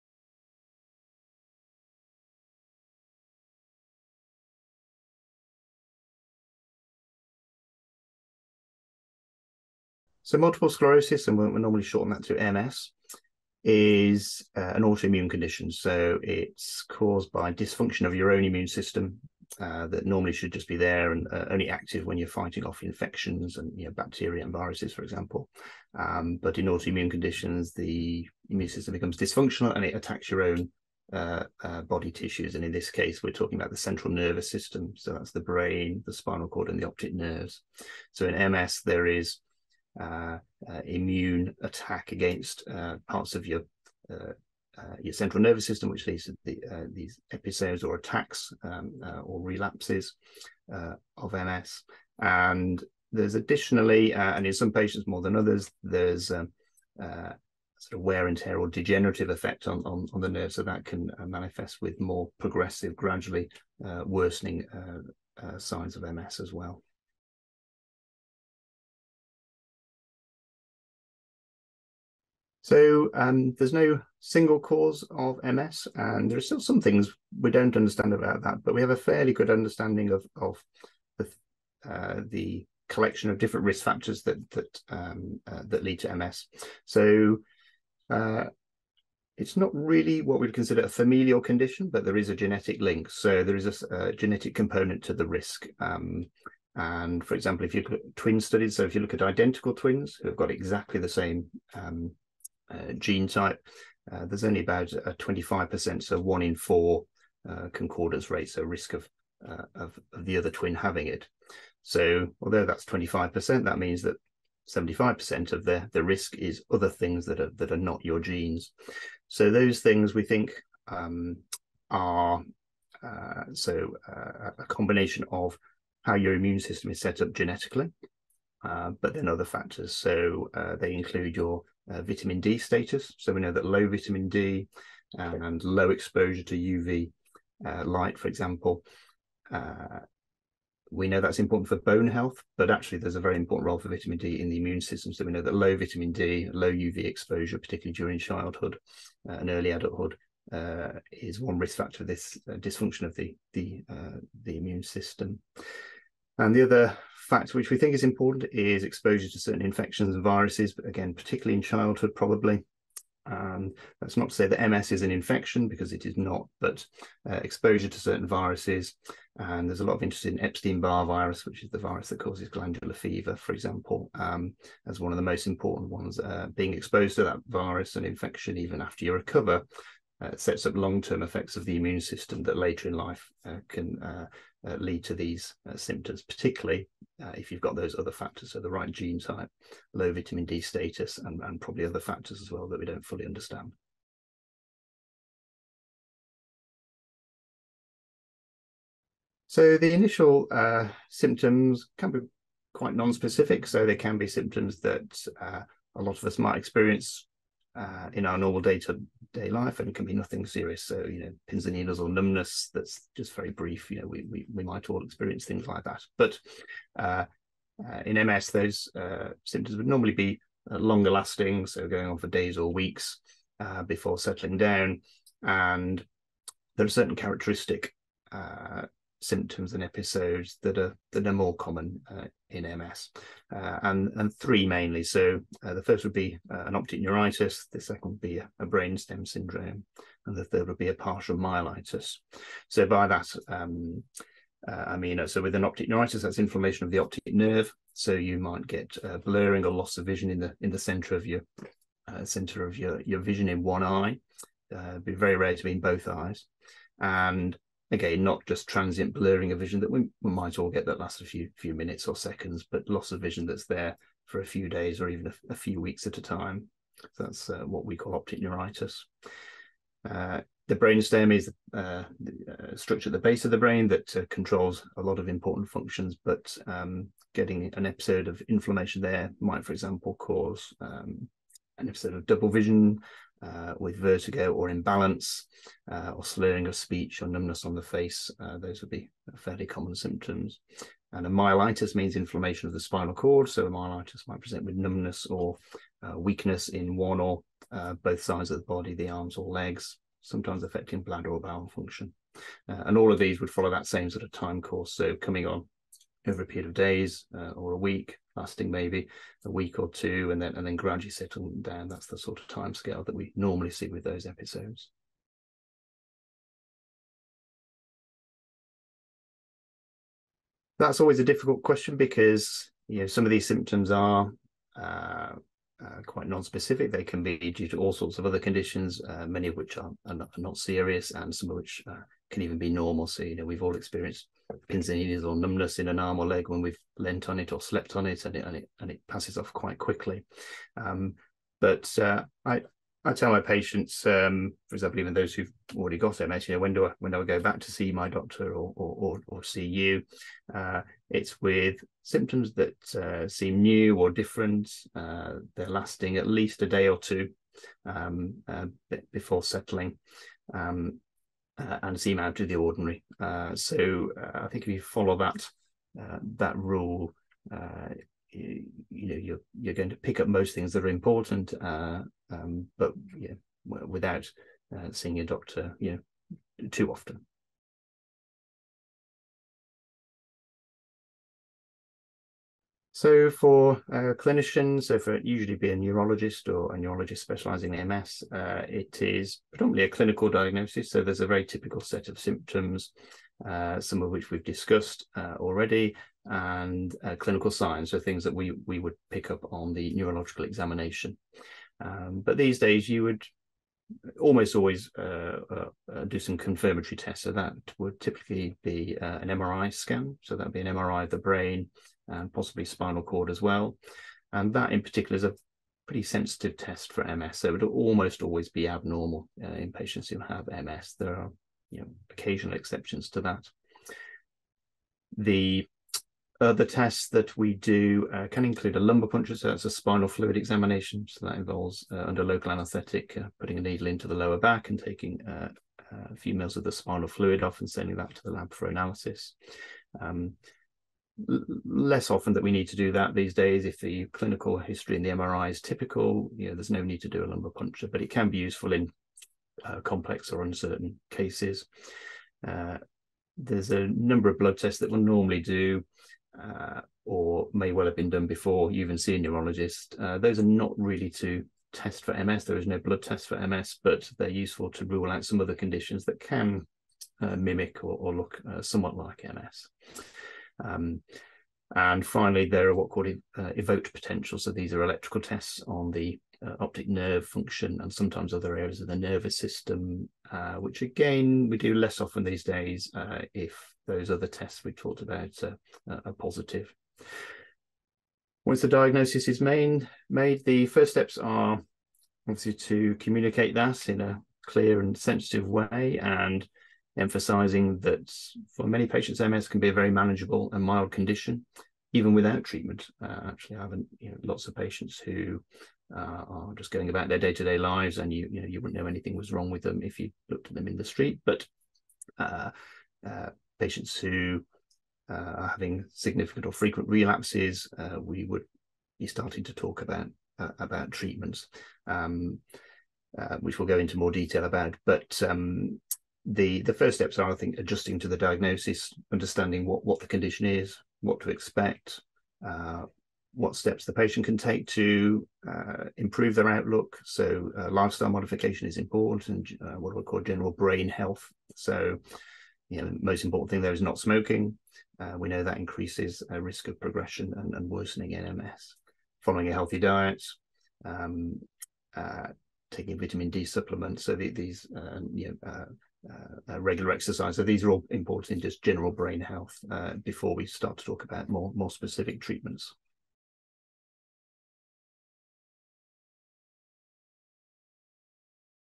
What are the early signs of multiple sclerosis? - Online interview
In this interview, Dr Martin Wilson, a highly revered consultant neurologist, details what multiple sclerosis is, and outlines what the early warning signs of the debilitating illness are.

00:00 What is multiple sclerosis?
01:08 What causes multiple sclerosis? Does it run in families?
03:14 What are the early signs of multiple sclerosis?
05:16 When should you see a doctor?
06:18 How is multiple sclerosis diagnosed?

If you are interested in booking a consultation with Dr Wilson, you can do so by visiting his Top Doctors profile